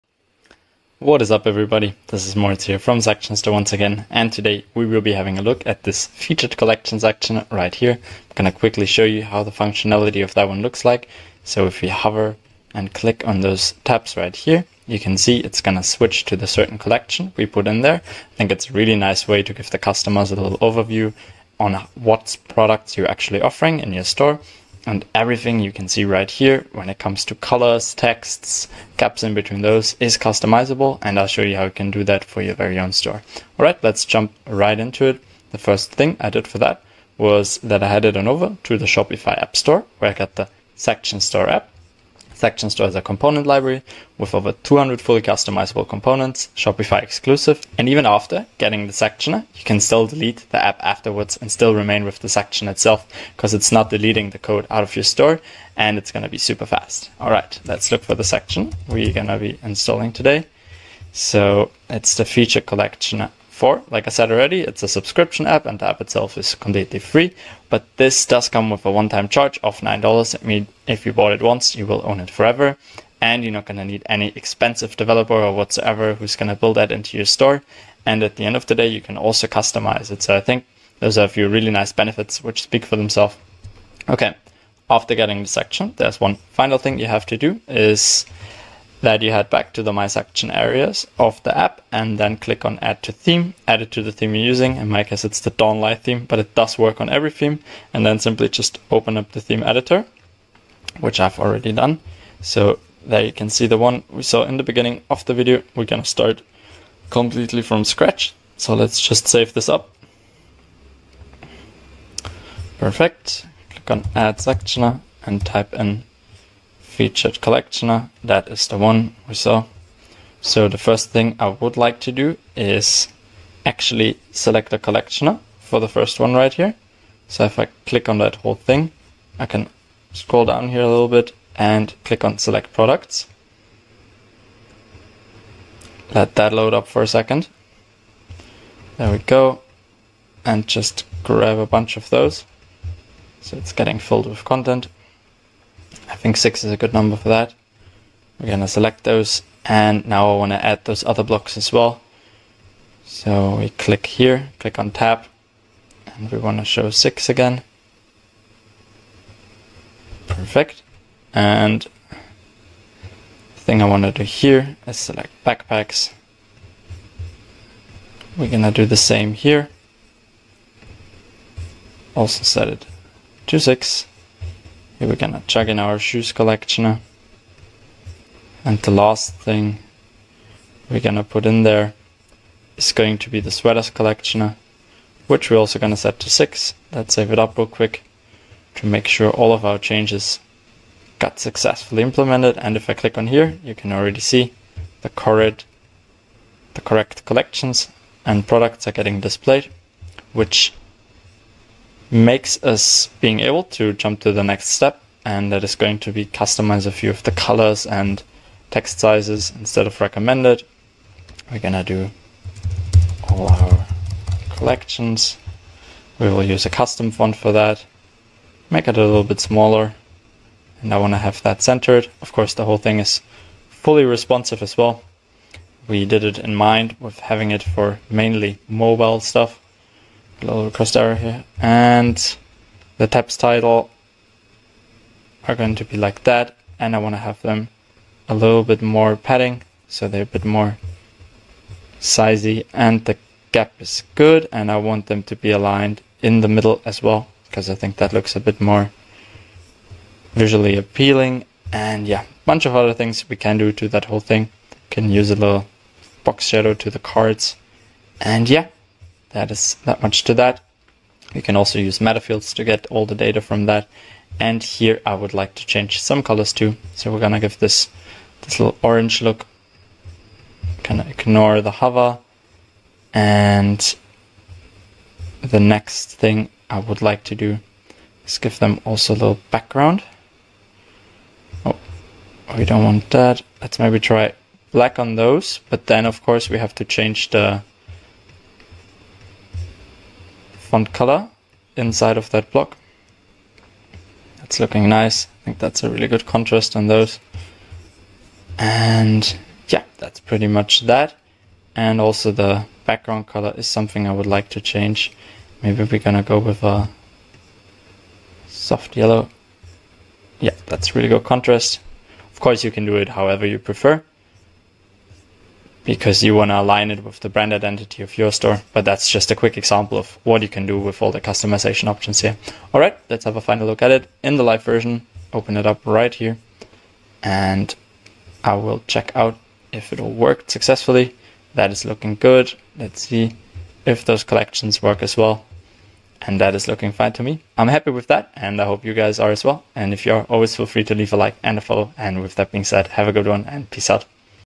Add Featured Products Collection to Shopify in 2024
Add this fully customizable Featured Products Collection to your Shopify.

Section store is a simple Shopify app that allows you to extend any theme with new theme sections.